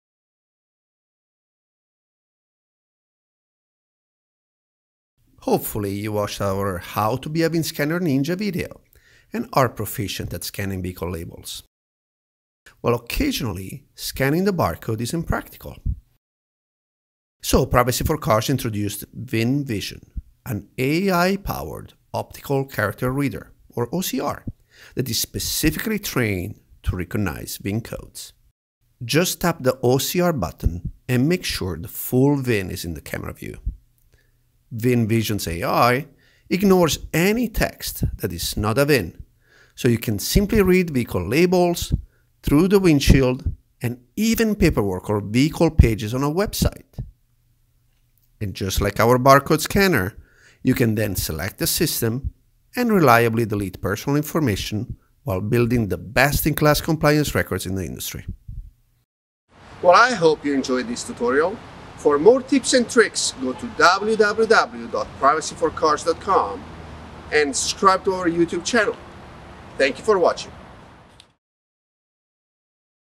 How to read a VIN with our AI-powered OCR
Sometimes reading a barcode or QR code is not practical. In those cases, you can use our VinVision Optical Character Recognition feature: an AI-powered OCR that we specifically trained to recognize VIN codes - and ignore all other text. Never enter a VIN manually again!